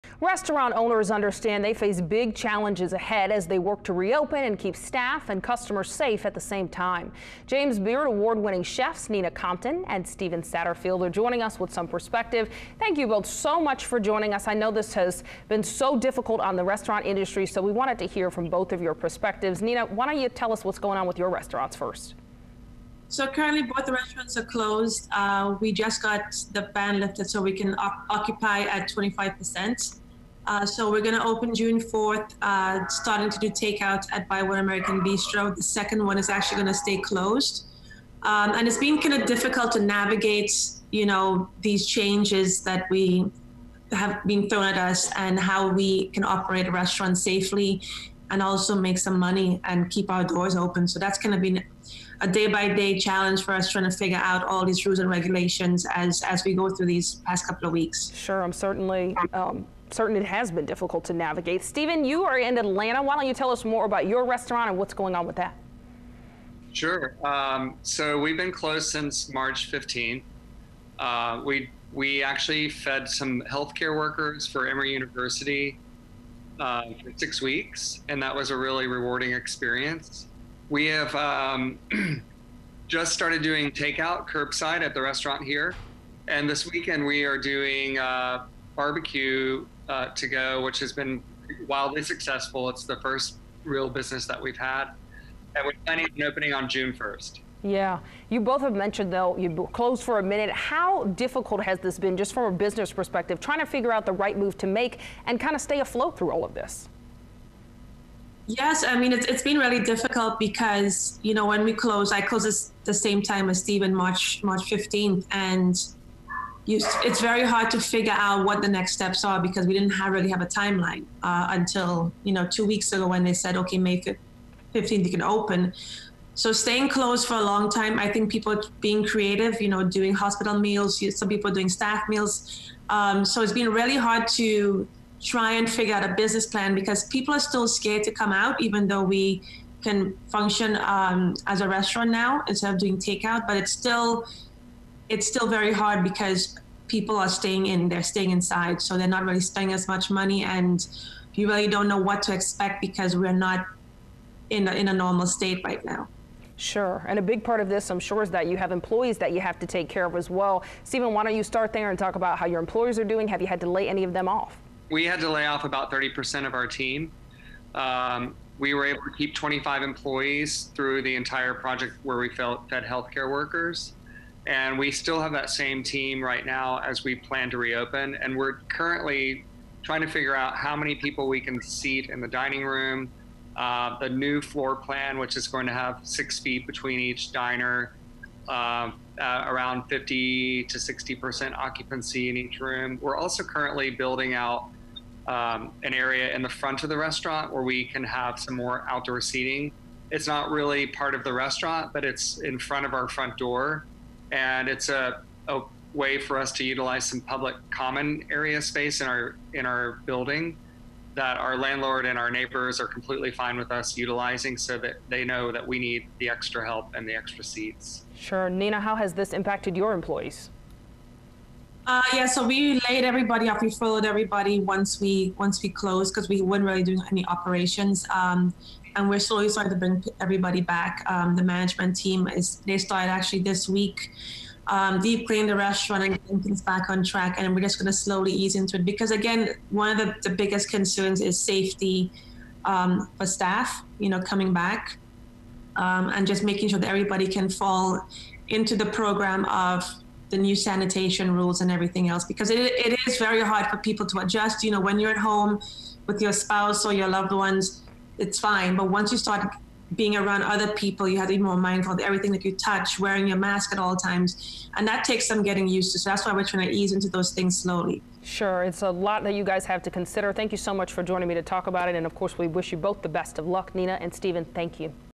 Award-winning chefs on COVID-19 reopening challenges in food industry
James Beard Award-winning chefs Nina Compton and Steven Satterfield talk about the difficulty navigating how to run their restaurants safely during COVID-19.